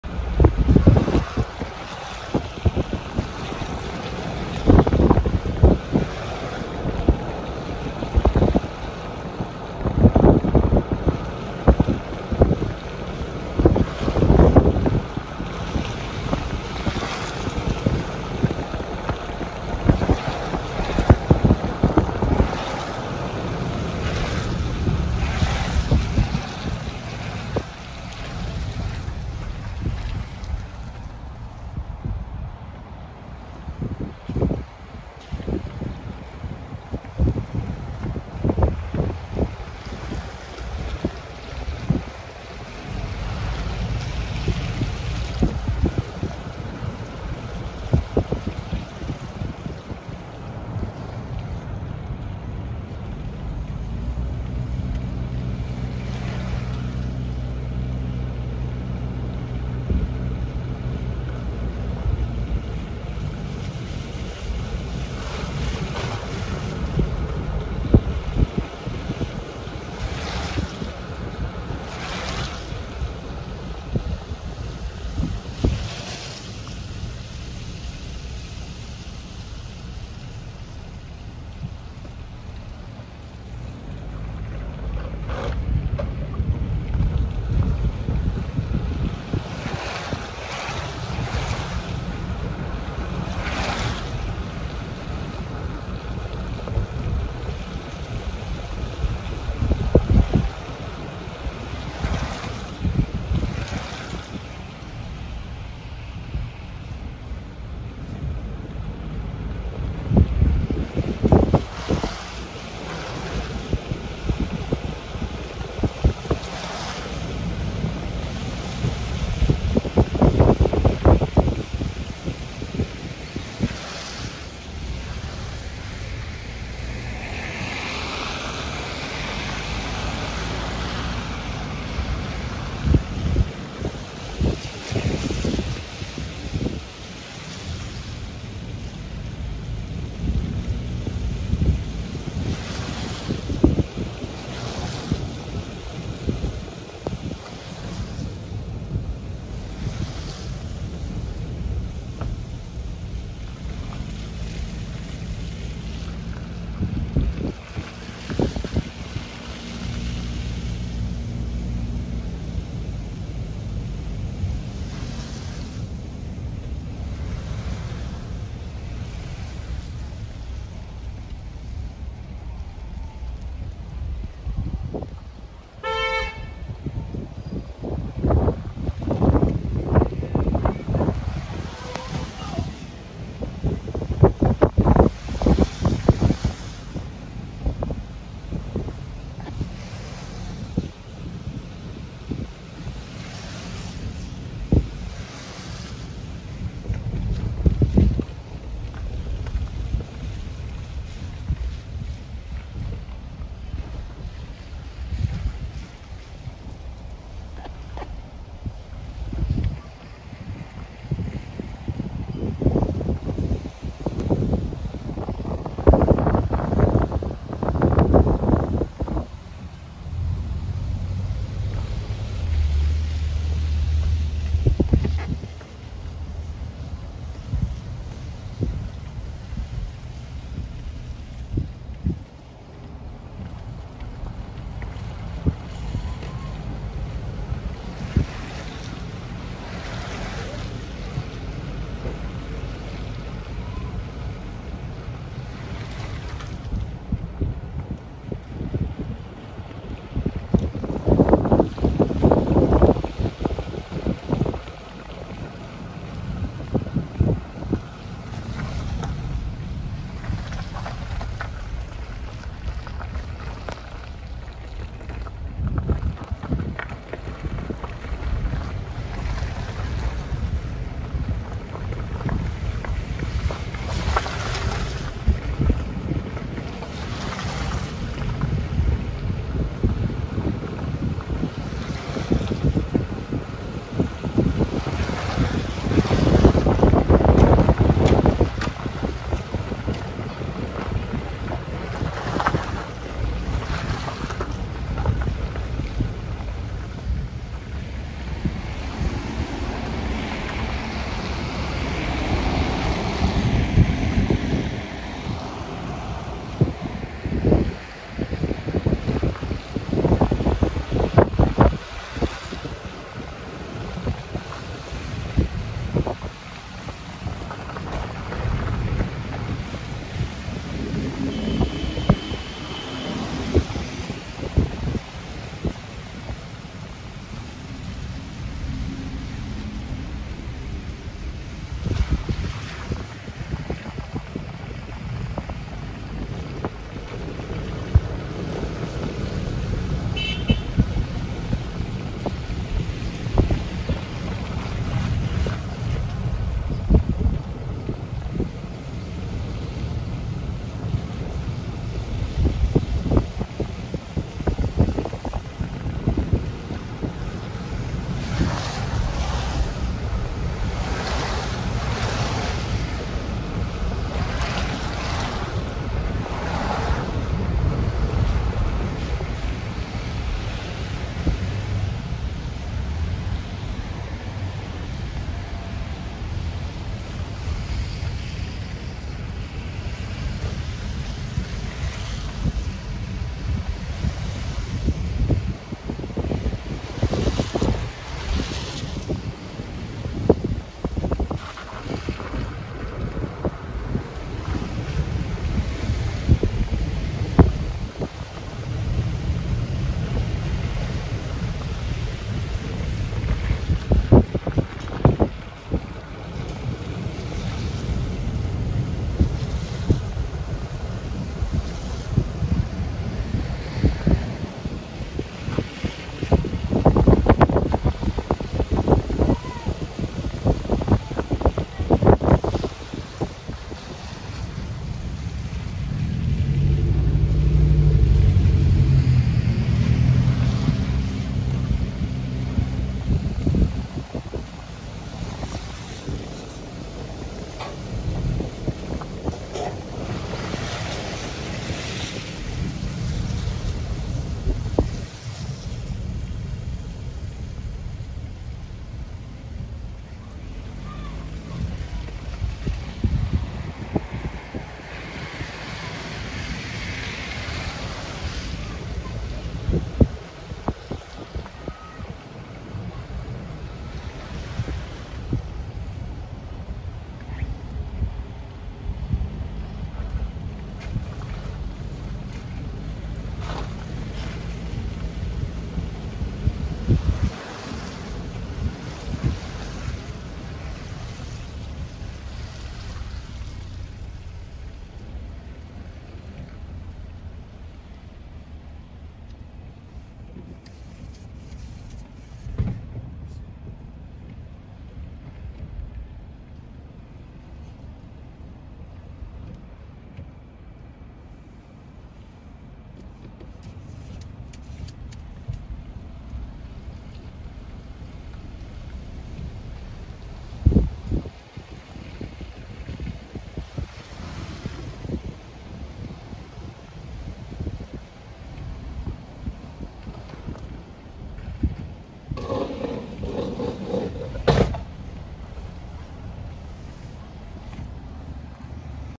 Snowy Adventure: Navigating Staten Island, NYC During an Arctic Blast
This video showcases a thrilling and challenging journey through Staten Island, New York City, amidst a fierce arctic blast. Viewers will experience the beauty and severity of a snow-covered landscape as we navigate slippery roads, snowdrifts, and icy conditions. Expect stunning visuals of winter in the city, along with practical tips for driving safely in extreme weather. Join us for a unique glimpse into how New York City endures and adapts during a harsh winter spell.